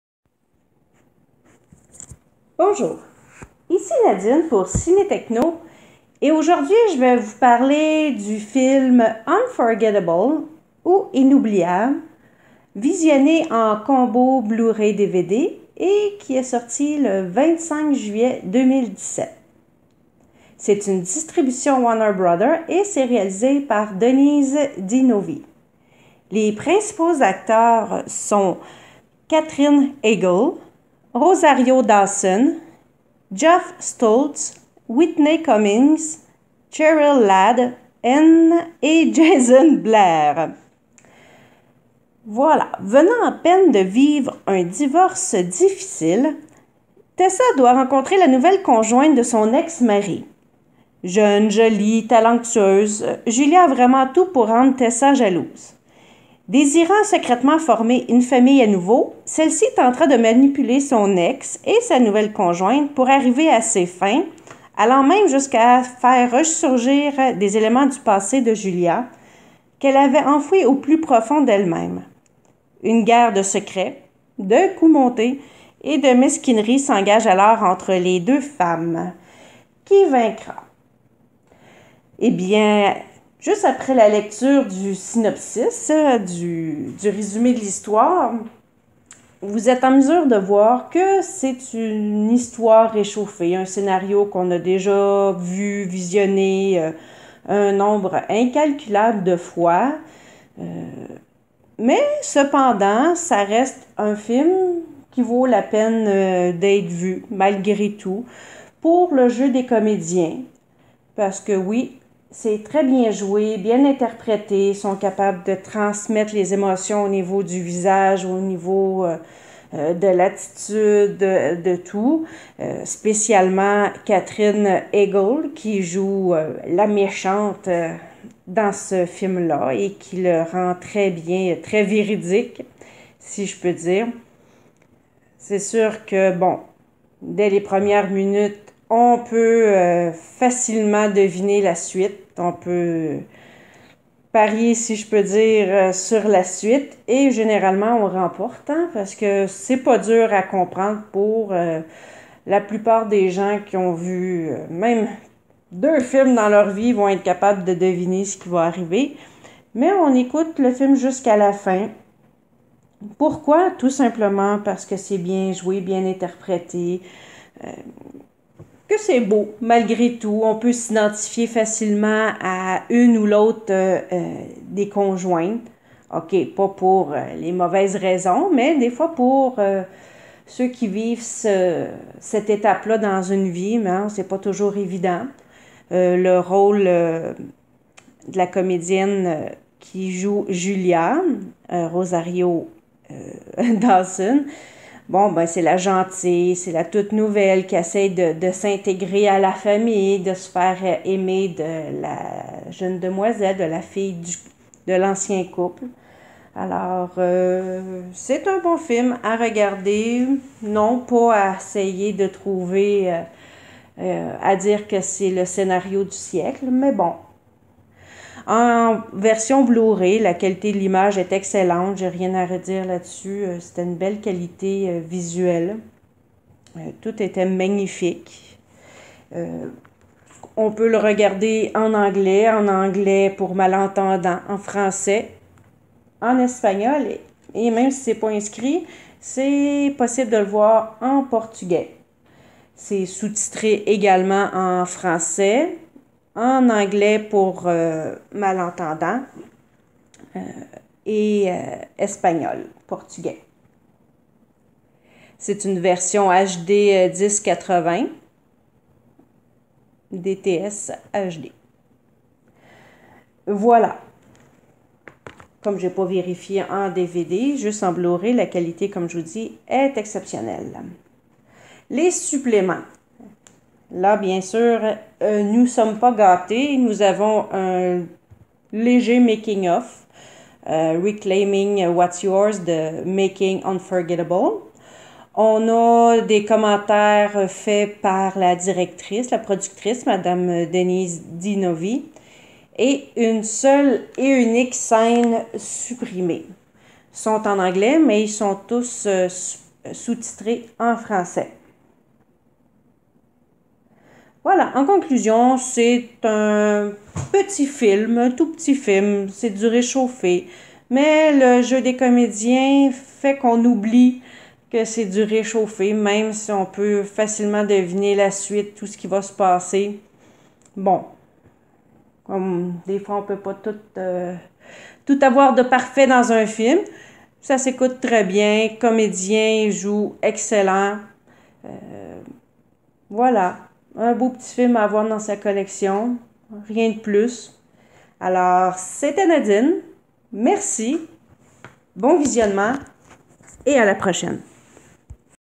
Critique du film Unforgettable(Inoubliable) en combo Blu-ray/DVD
Critique du film Unforgettable(Inoubliable) en combo Blu-ray/DVD par Nadine pour Réalisé par Denise Di Novi et mettant en vedette Katherine Heigl, Rosario Dawson, Geoff Stults, Whitney Cummings, Cheryl Ladd et Jayson Blair. Distribué par Warner Bros, vous pouvez vous le procurer en magasin dès le 25 juillet 2017.

Venant à peine de vivre un divorce difficile, Tessa doit rencontrer la nouvelle conjointe de son ex-mari. Jeune, jolie, talentueuse, Julia a vraiment tout pour rendre Tessa jalouse. Désirant secrètement former une famille à nouveau, celle-ci tentera de manipuler son ex et sa nouvelle conjointe pour arriver à ses fins, allant même jusqu'à faire ressurgir des éléments du passé de Julia, qu'elle avait enfouis au plus profond d'elle-même. Une guerre de secrets, de coups montés et de mesquineries s'engage alors entre les deux femmes... Qui vaincra?